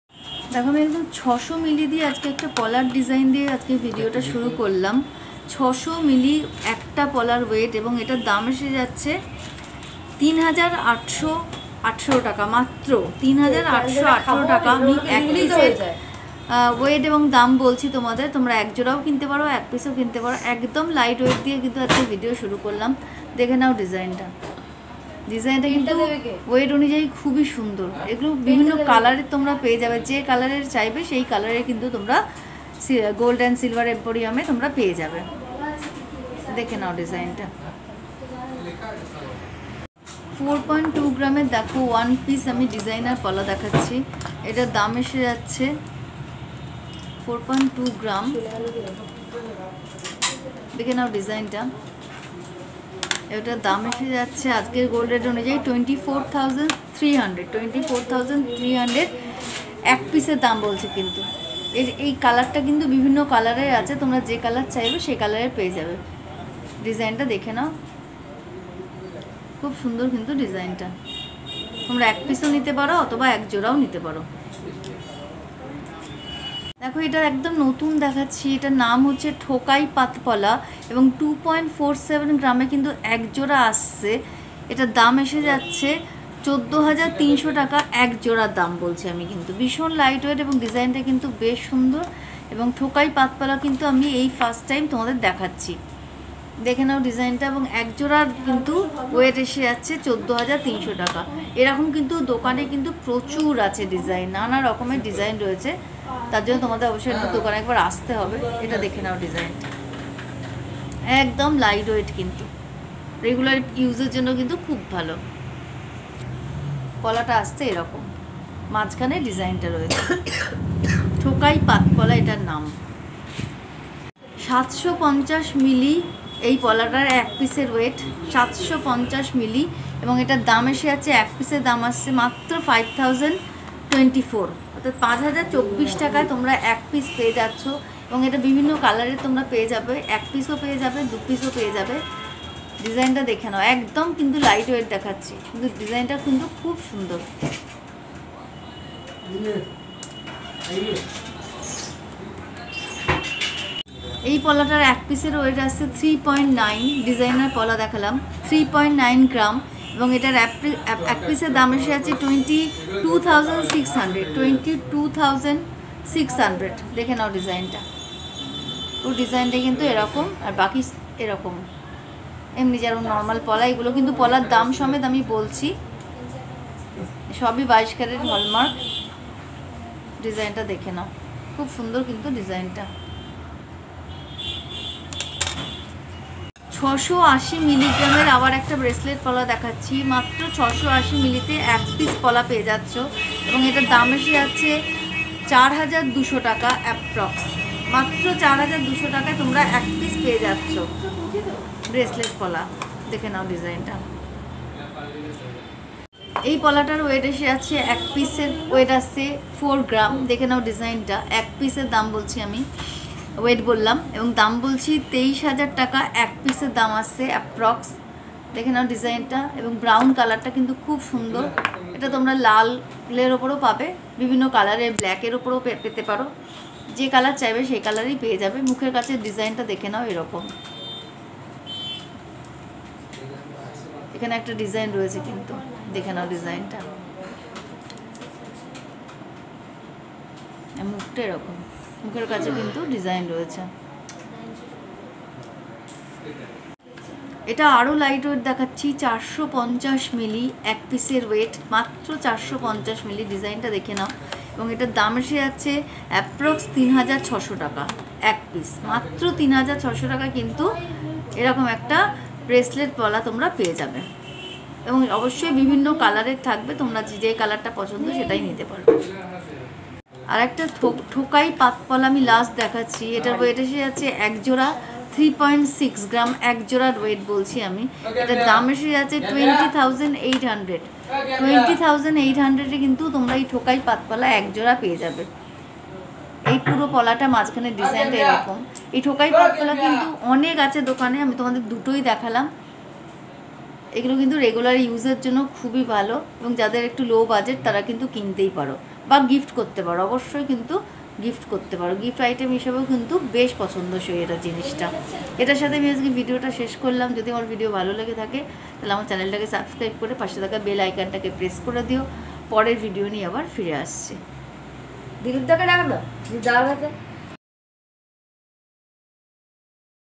Light Weight Gold Pola Badhano || Thokai Pat Pola || Pola Badhano starts from 600 milligram
Hi Everyone - Today I visited Gold and Silver Emporium, B B Ganguly Street, Kolkata
rojnamchar Patay